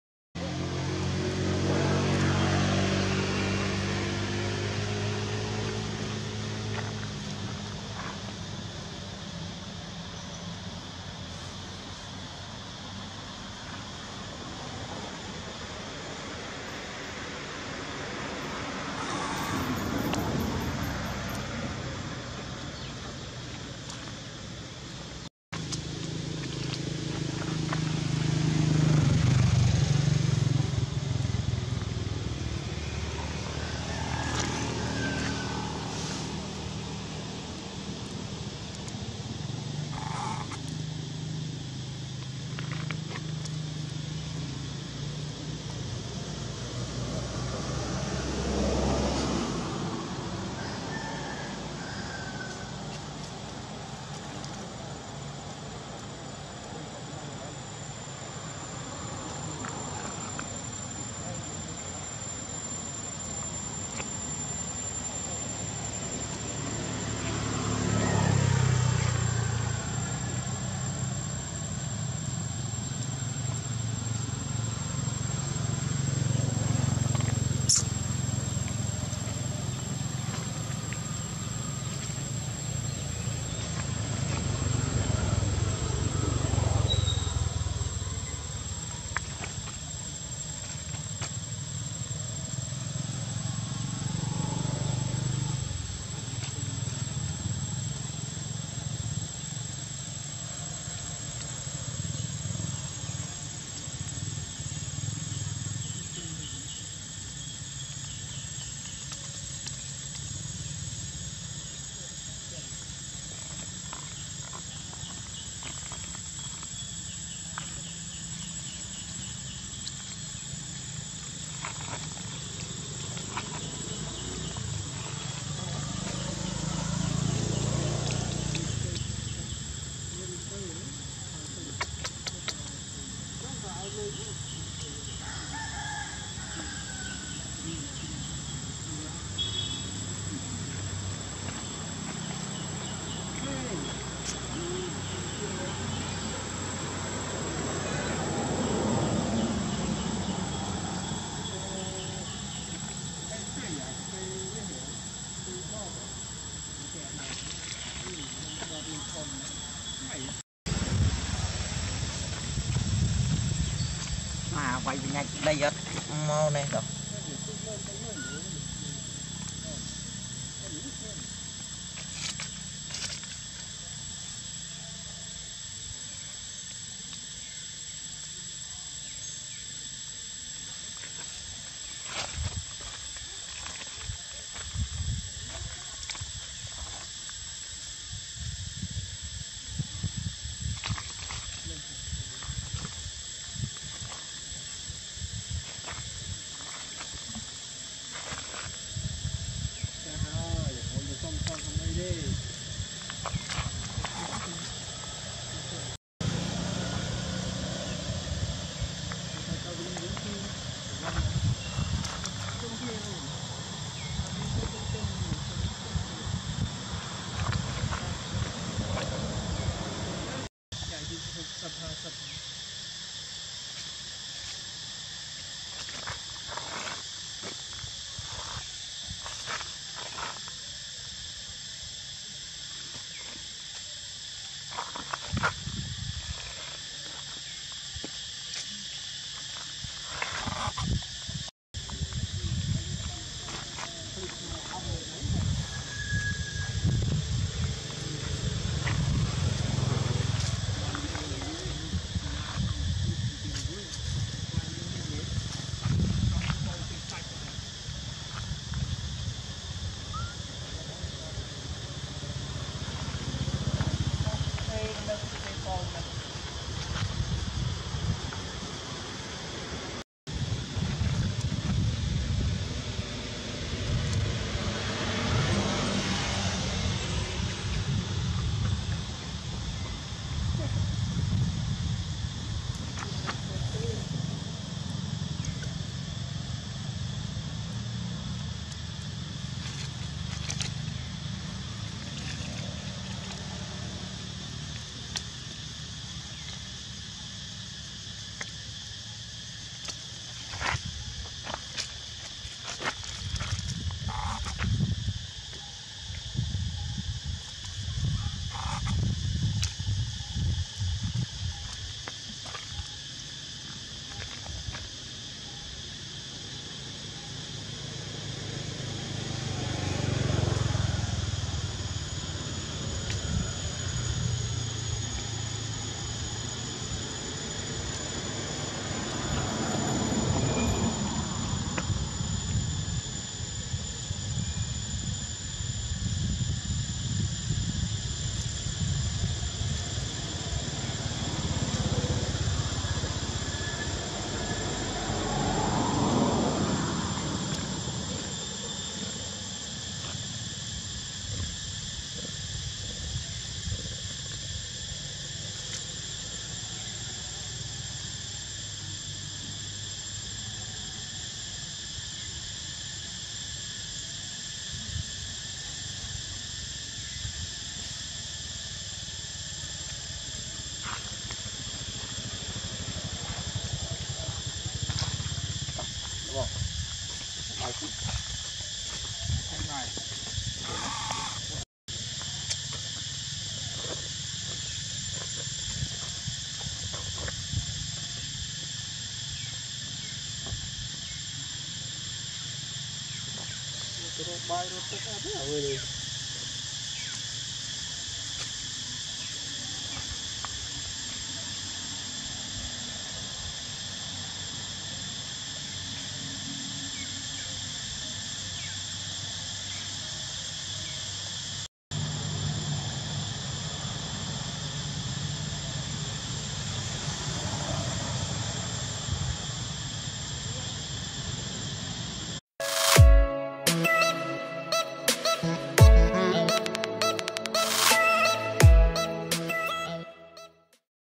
Monkey Amara Never Forget To Suckle Adorable Annie
Thanks for loving animals especially monkeys. I film monkeys, protect them, and feed them. I love monkey baby and their family.